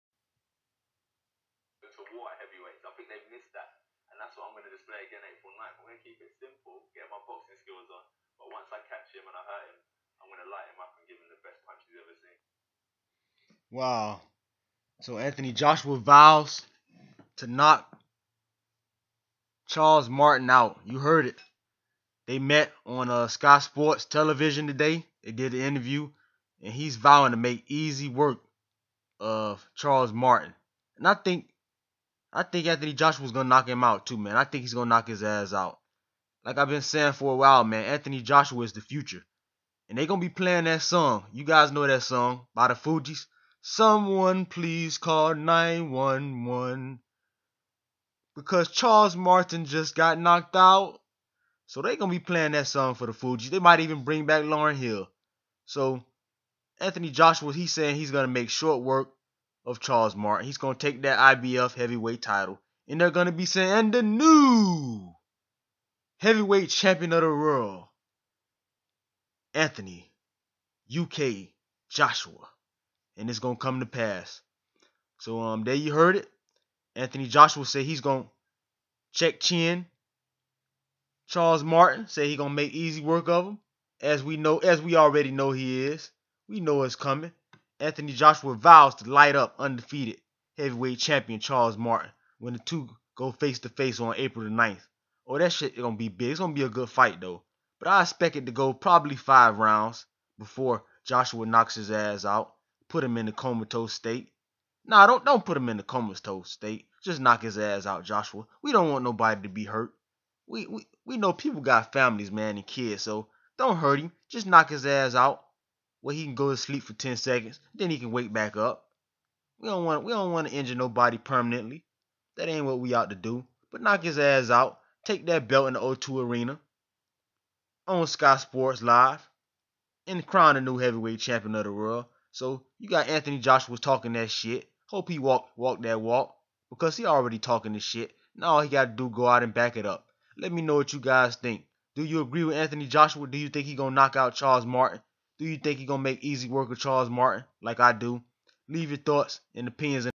ANTHONY JOSHUA AND CHARLES MARTIN FACE TO FACE NEARLY COME TO BLOWS, JOSHUA VOWS KNOCKOUT
Anthony Joshua says he will 'light up' undefeated IBF heavyweight world champion Charles Martin, when the two face each other on April 9.

Joshua (15-0-KO15) challenges Martin (23-0-1-KO21) for the IBF heavyweight belt in what will be the British boxer's first shot at a title fight.

The bout at the O2 Arena, which will be shown live on Sky Sports Box Office, is Martin's first title defence since defeating Ukranian fighter Vyacheslav Glazkov in January.

Anthony Joshua and IBF Champion Charles Martin come face to face in order promote their April 9th fight in London,UK. Anthony Joshua is a gold medalist and this will be Charles Martin first defense of his world heavyweight title

Anthony Joshua vows to knock out undefeated IBF heavyweight champion Charles Martin